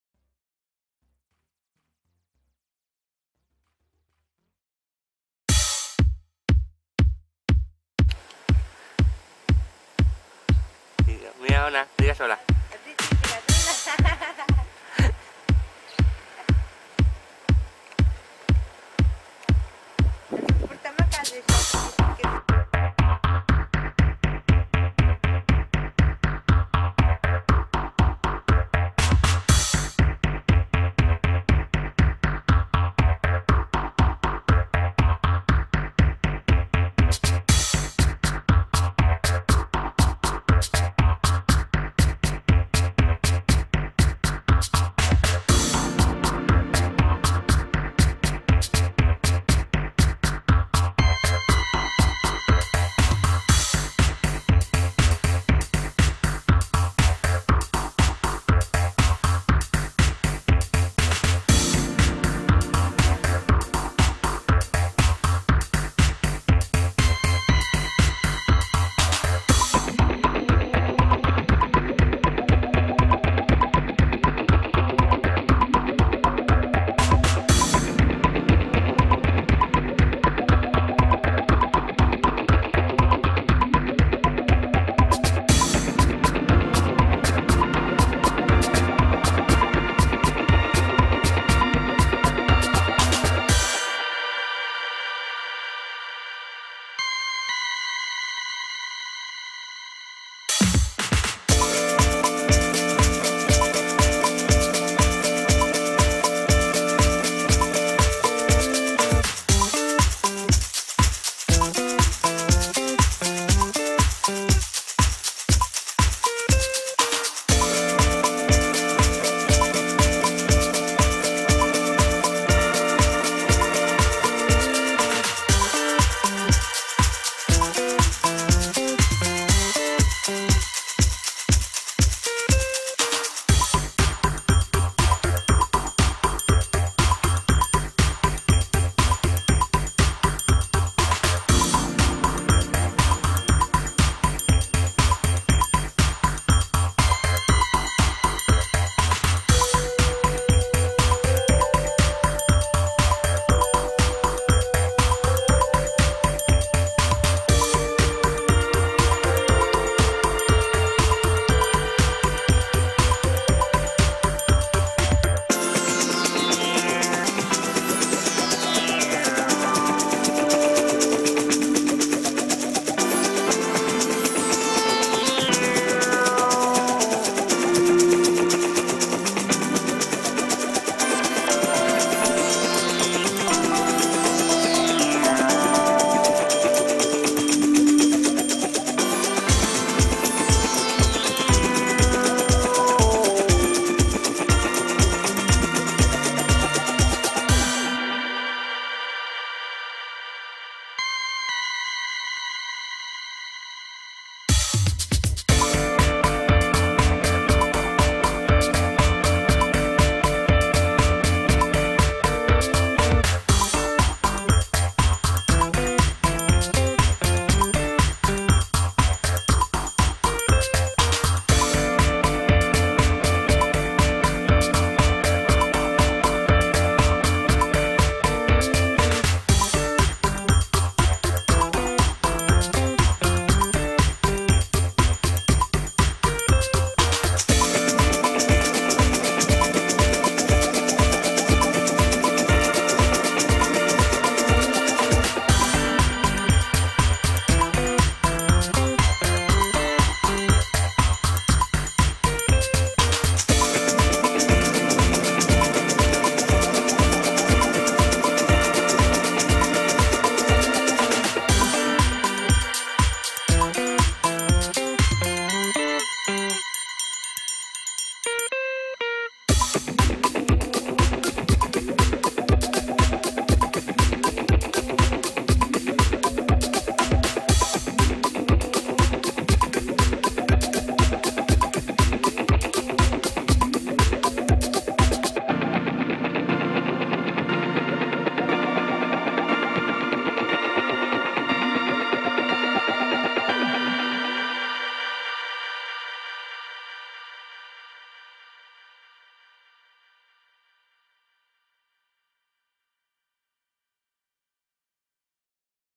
Visita Al Roc de sant gaieta
Hola amigos y amigas hoy hemos ido a visitar el roc de sant gaieta,

Facebook: Nos Gusta Viajar en familia